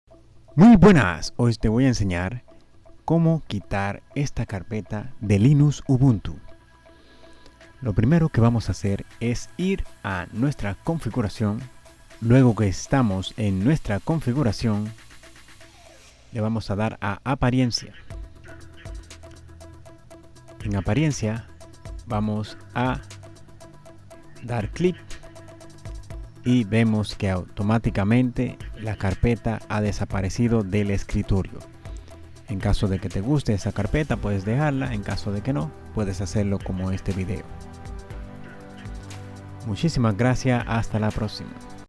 Como quitar la carpeta Personal en linux ubuntu 22.04 LTS
Hoy te voy a mostrar como quitar esa carpeta de linux ubuntu 22.04 lts de tu escritorio

00:00 explicacion
0:10 ir a configuracion
0:18 luego le daremos a apariencia
0:25 al final carpeta personal